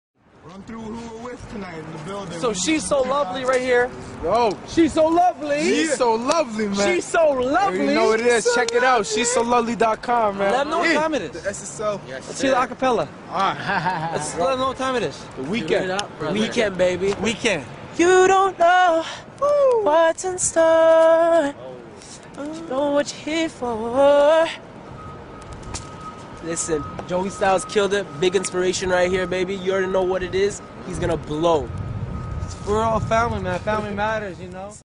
The Weeknd Before Fame!
A video of The Weeknd singing a little acapella before he achieved his weel deserved mainstream fame! This video is not mine and is of the original creator/first uploaded. I have just uploaded this for others see. Thanks